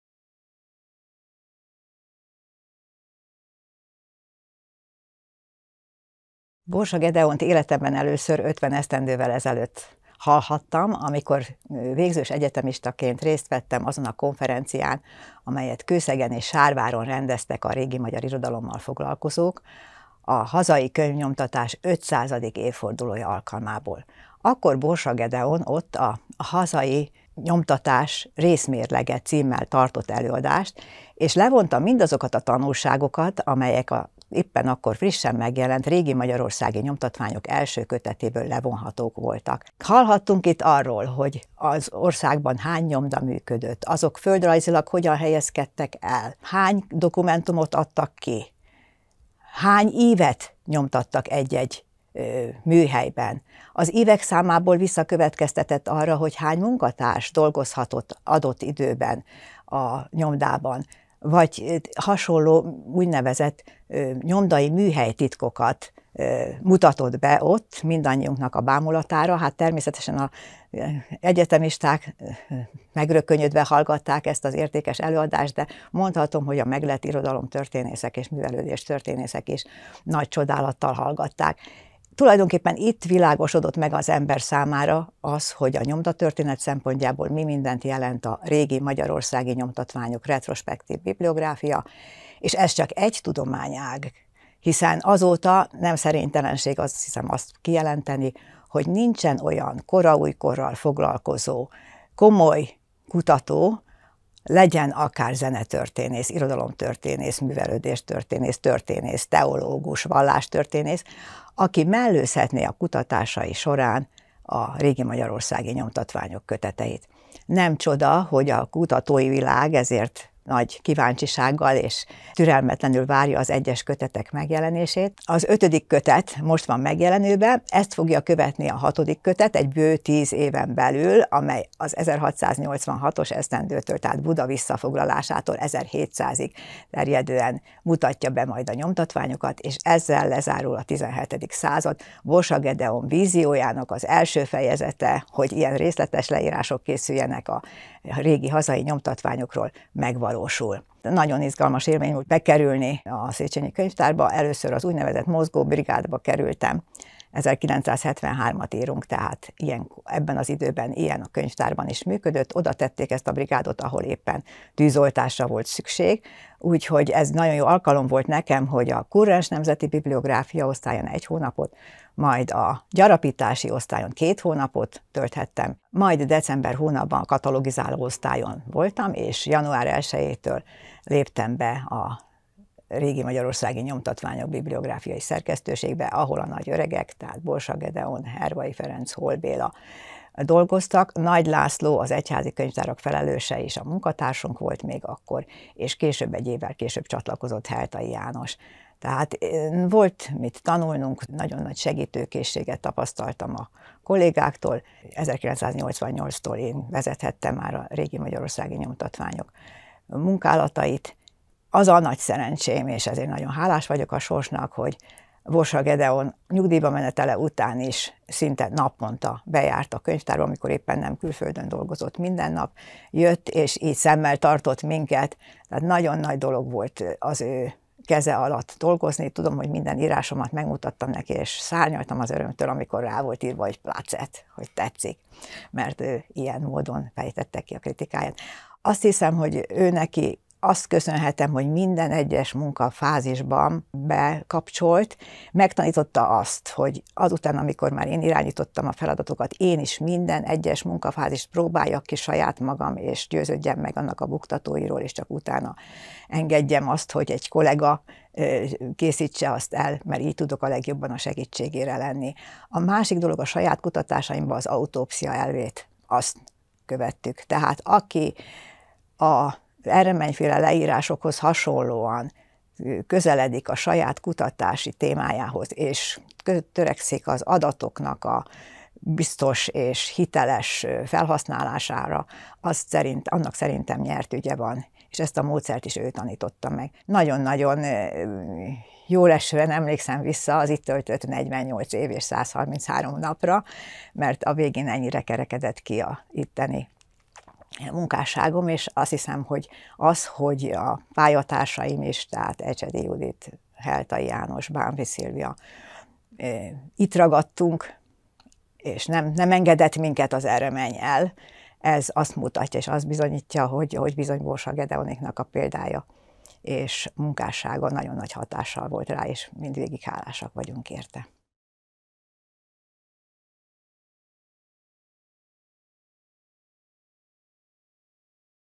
P. Vásárhelyi Judit Borsa Gedeonról – Borsa 100 – RMNY V. 2023. október 11.
P. Vásárhelyi Judit, a Régi Magyarországi Nyomtatványok irodalomtörténész kutatója, szerkesztője, az OSZK volt tudományos munkatársa mesél Borsa Gedeonhoz és az RMNY csoporthoz fűződő emlékeiről.

Beszél a kezdetekről, személyes indíttatásáról, amikor fiatal egyetemistaként először hallhatta Borsa Gedeont a hazai könyvnyomtatás 500. évfordulóján tartott tudományos konferencián előadni (1973). Megtudhatjuk tőle, milyen kutatási elvek és munkafázisok határozták meg az RMNY mindenkori munkálatait és hogy milyen korszakhatárral zárul majd az utolsó, tervezett RMNY kötet.

Borsa Gedeon születésének századik évfordulóján, 2023. október 11-én jelenik meg az általa elindított retrospektív nemzeti bibliográfia, a Régi Magyarországi Nyomtatványok (1671–1685) V. kötete. A könyv megjelenését megelőzően minden hónap 11-én egy-egy „RMNY-történettel” emlékezünk a jeles bibliográfusra.

Borsa 100 – RMNY V.

2023. október 11.